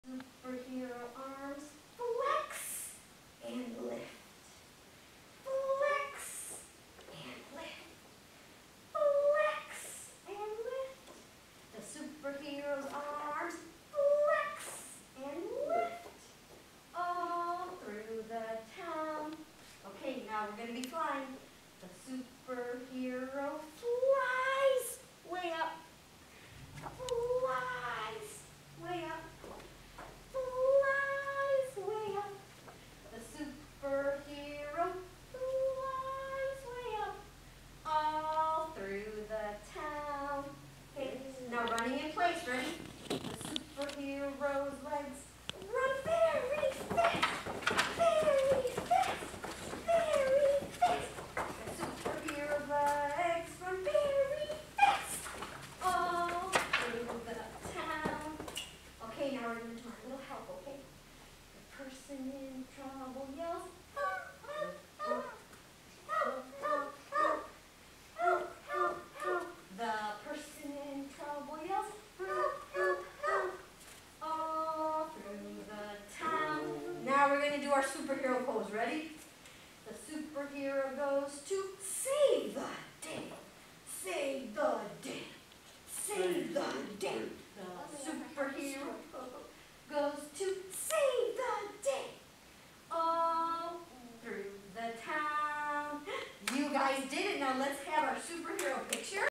Superhero Camp October 2021
Miss Diana leads her superheroes in song after they leapt tall buildings in a single bound, jumped through rings of fire (pretend), lifted weights to the sky and so much more.